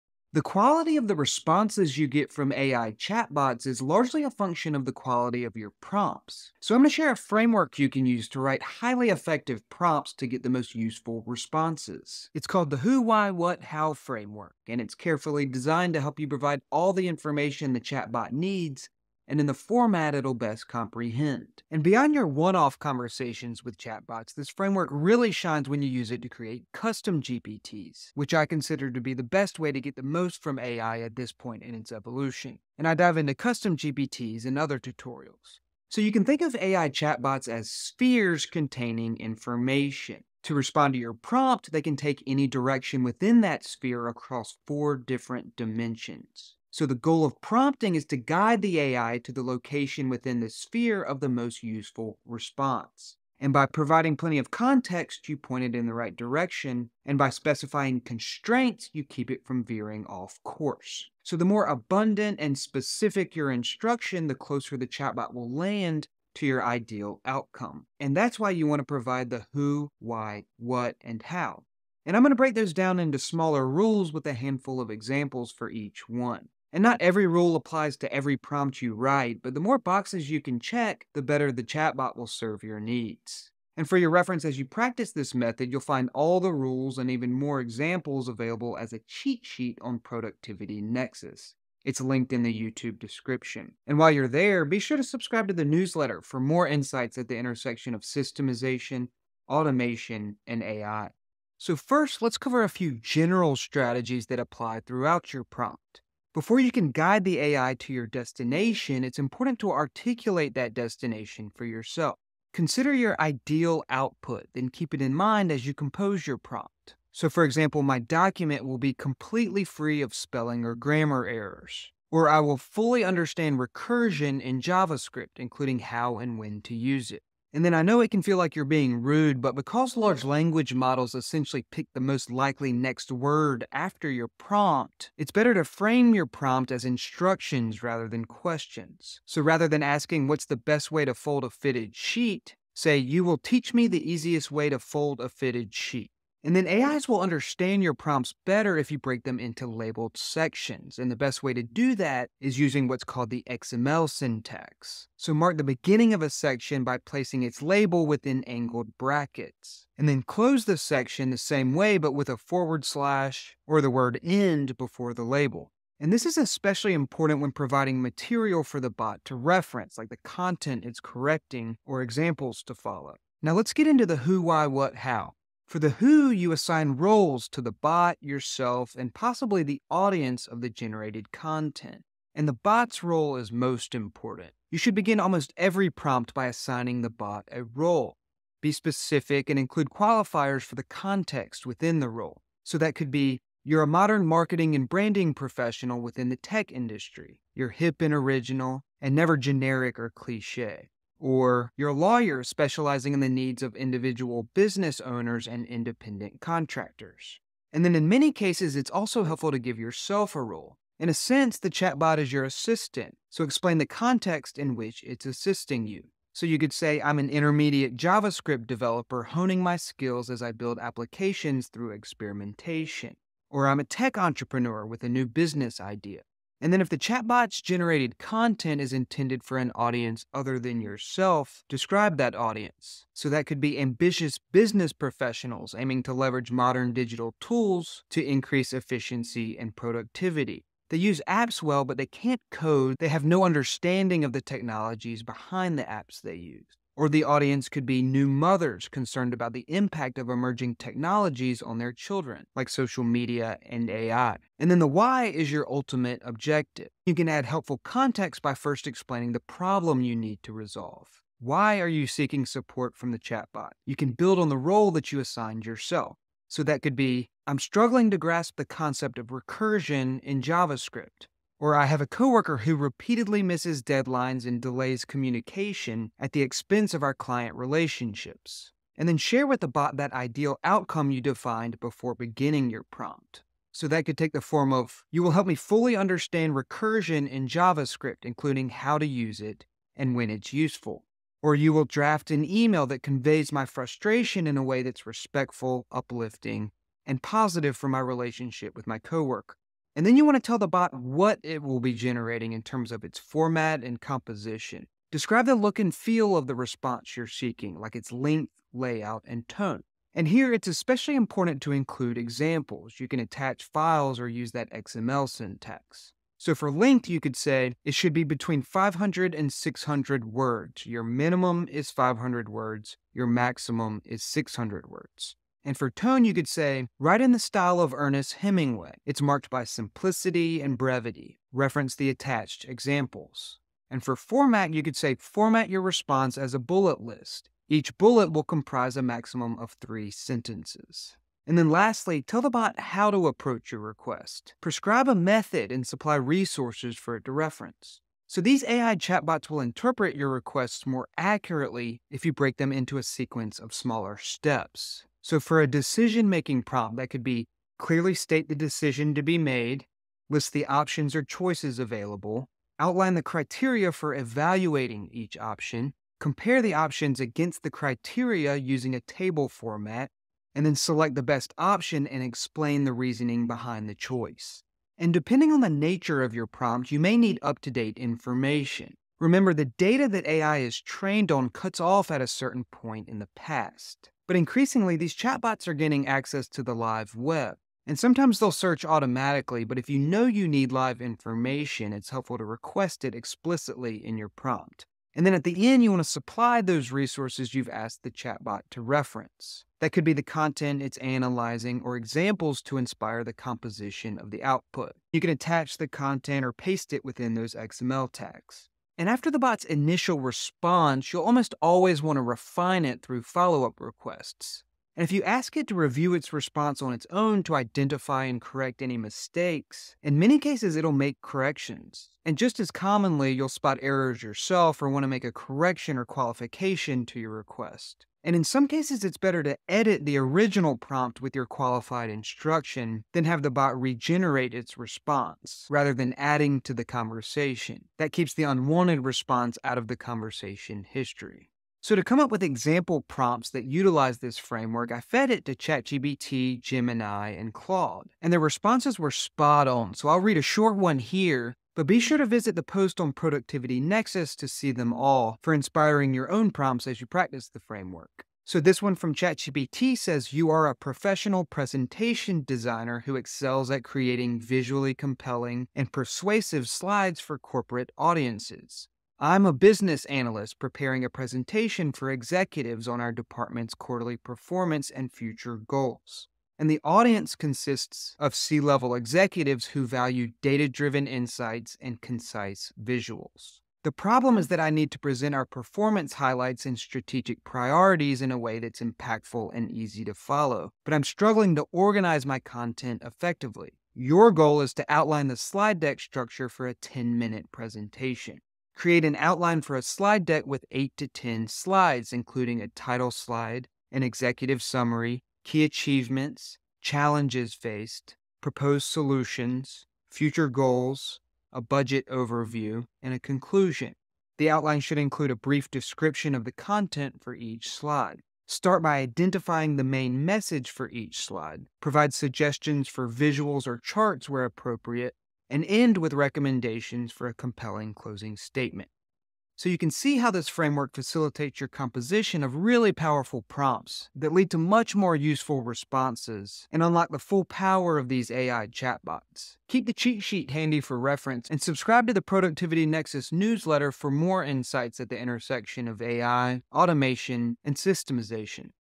Get 10x Better AI Responses with the WWWH Prompting Framework
The quality of your responses from ChatGPT, Claude, Gemini, and other AI chatbots depends on the quality of your prompts. The simple WWWH framework ensures you provide the information the bot needs, in a format it will comprehend.

00:36 — The Goal of Prompting
01:42 — Universal Prompting Strategies
02:54 — Who: Assign Roles
04:15 — Why: Identify the Objective
05:00 — What: Describe the Composition
05:42 — How: Supply the Method + Resources
06:49 — Follow-Up
07:22 — Example